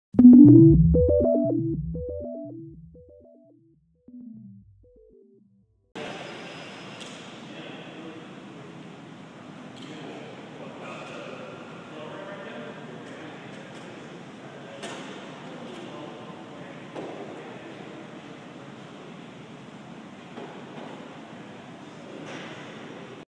Delta Fountains Water Screen
This Water Screen was for our 250 W. 57th Street project in Manhattan, NY.